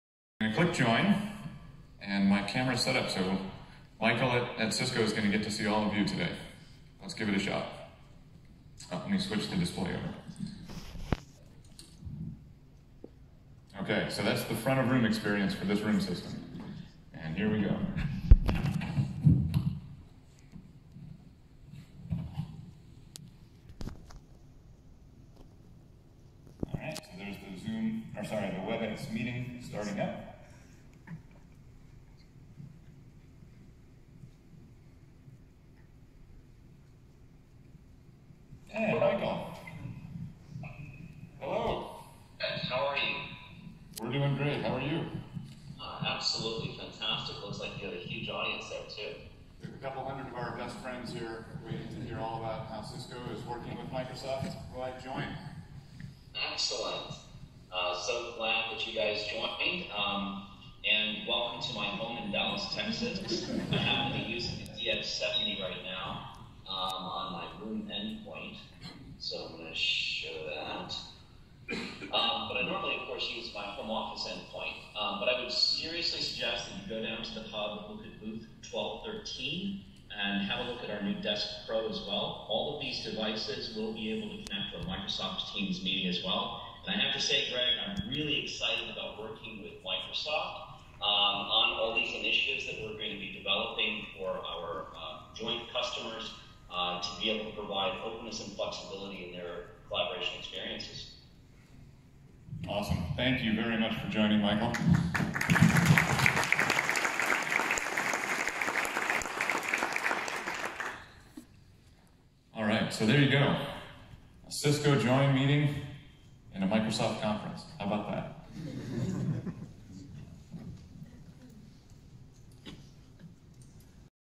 Microsoft Teams Room system joining a Cisco Webex Meeting
Microsoft Teams Room system joining a Cisco Webex Meeting as Guest Join Access with a WebRTC browser